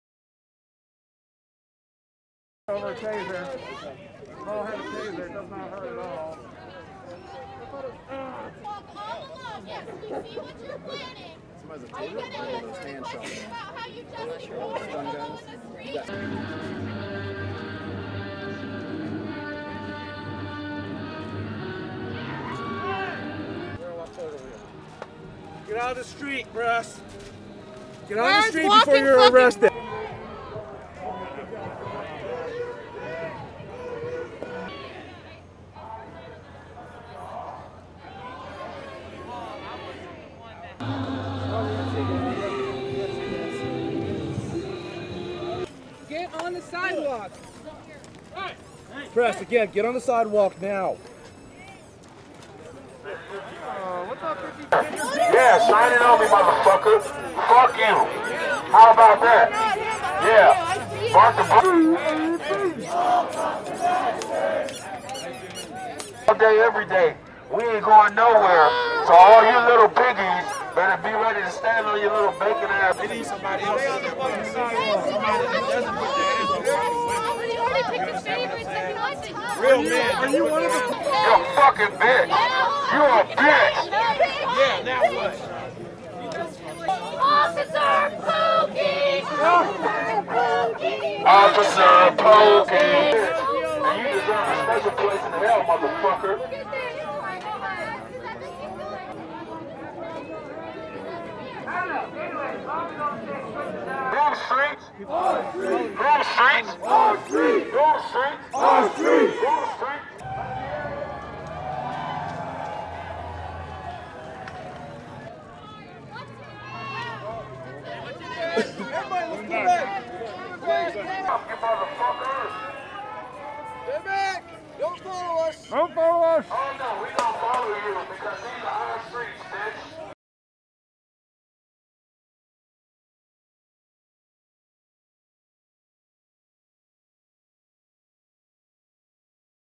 Portland marks 100 days since Black Lives Matter protests began
Police officers arrest Black Live Matters activists during another night of protests in Portland, Oregon. This weekend marks 100 days since protests erupted denouncing police brutality and racism.